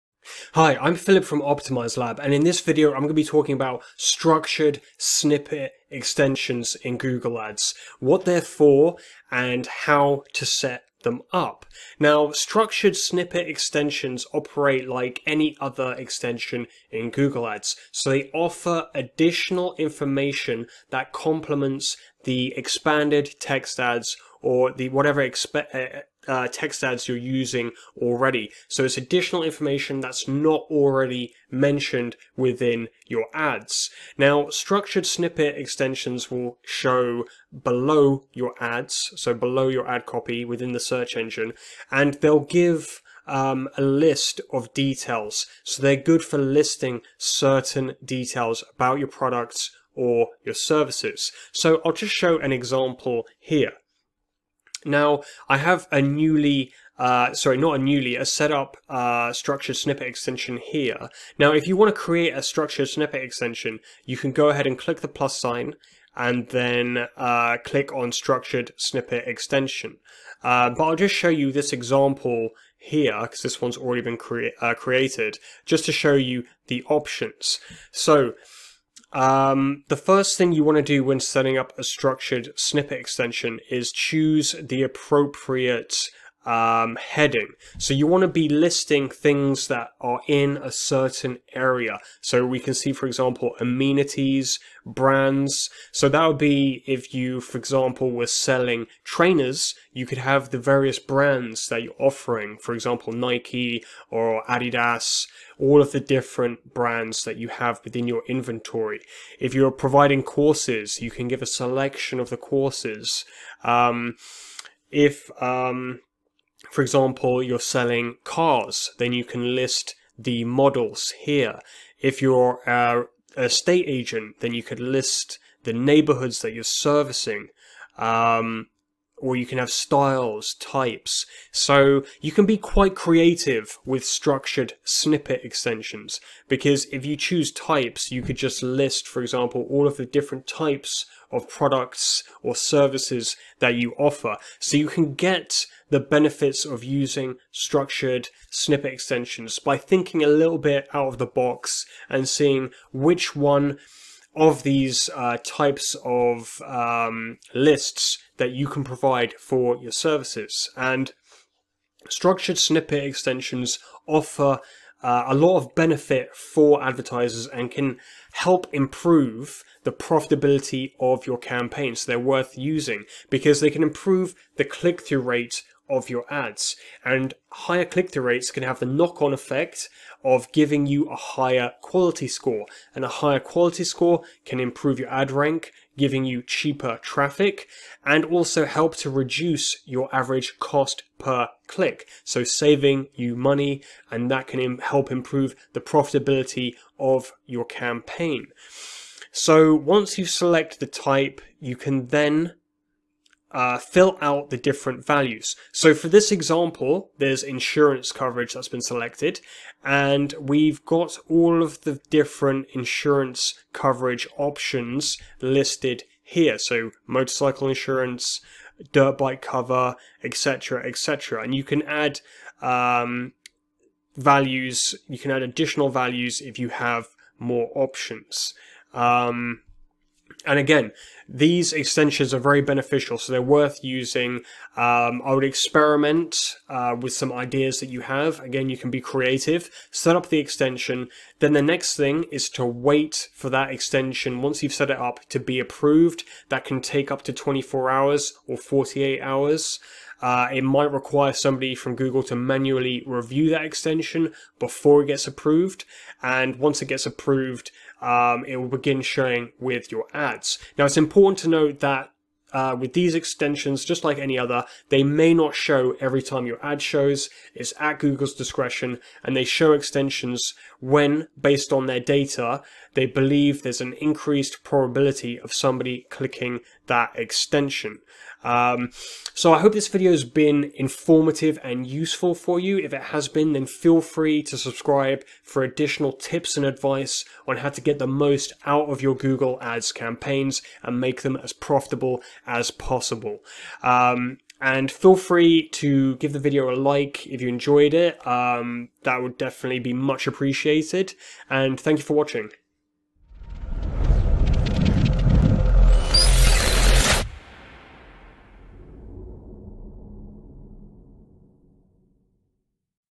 What Are Structured Snippet Extensions Google Ads
What Are Structured Snippet Extensions Google Ads - find out how to use Structured Snippet Extensions in your Google Ads campaigns 👇

👀 Links to The Most Watched Videos from This Channel 👀